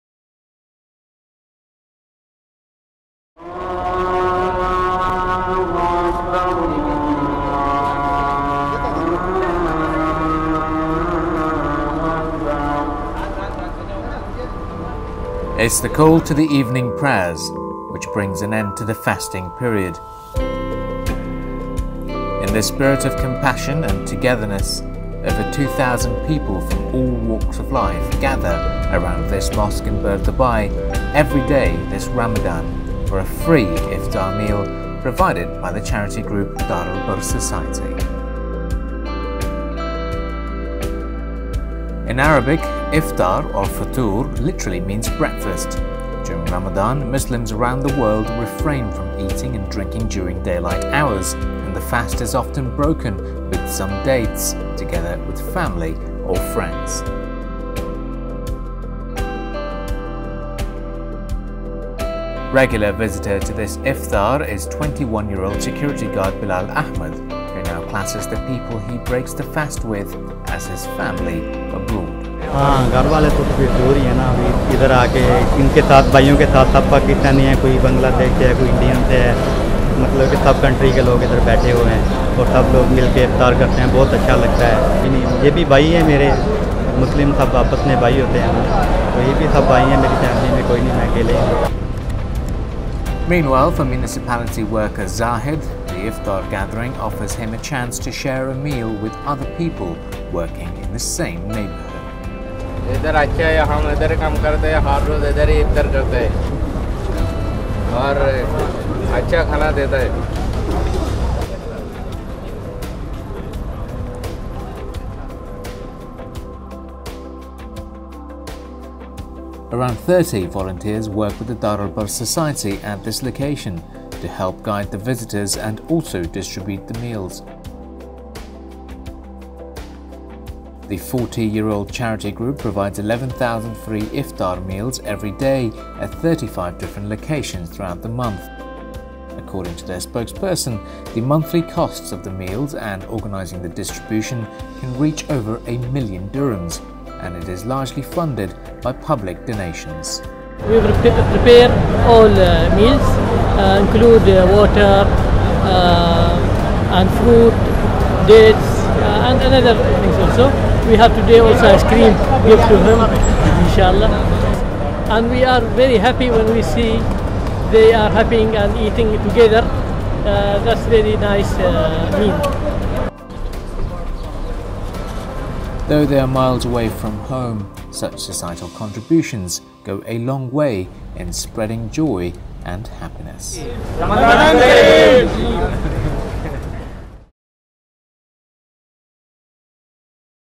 Iftar with Dar Al Ber Society
Over 2,000 people gather at the Ibrahim Khalil Mosque in Bur Dubai, where Dar Al Ber Society distributes free iftar meals.

Independent report produced by myself to showcase this initiative.